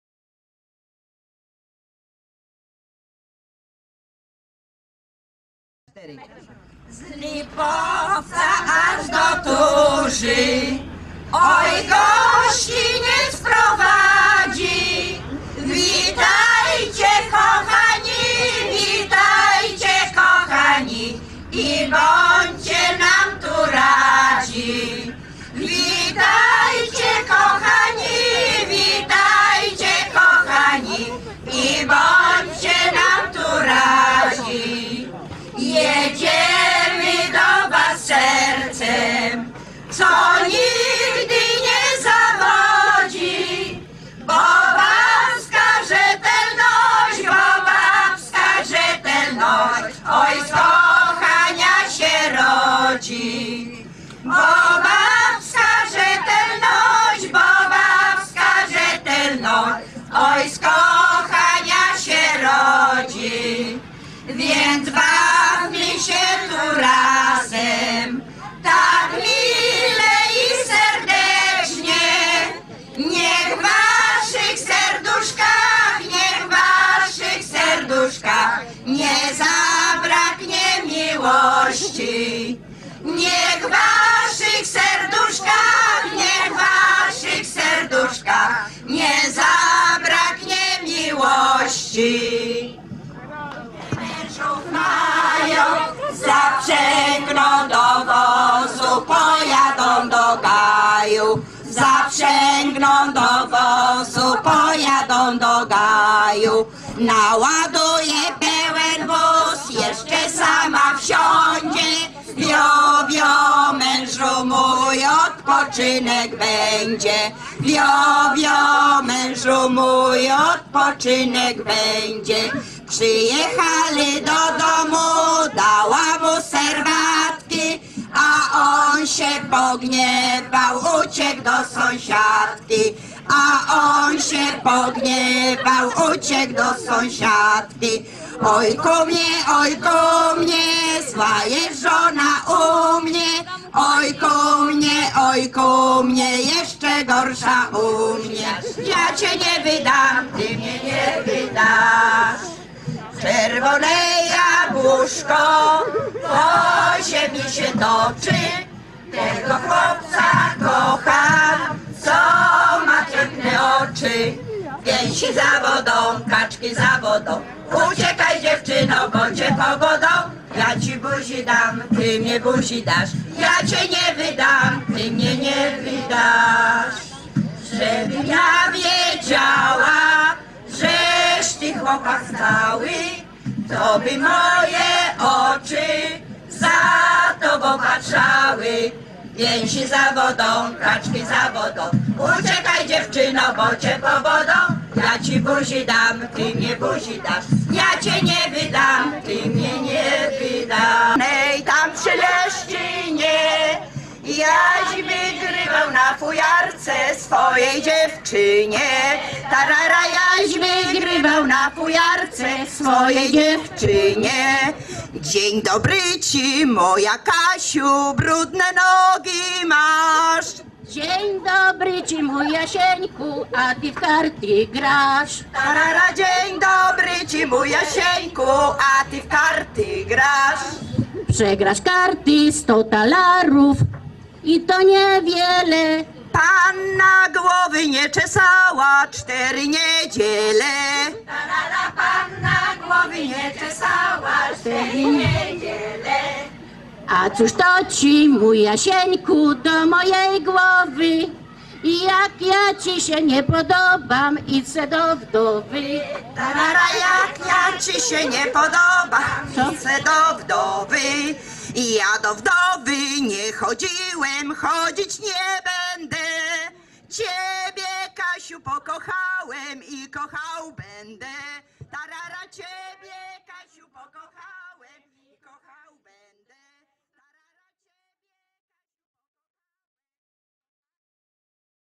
Festyn rodzinny - Turza Wielka 2011.1avi.mp4
30 lipca 2011 Turza Wielka - Festyn rodzinny. Występ zespołu "Klubu Seniora" działającego przy GCK w Lipowcu Kościelnym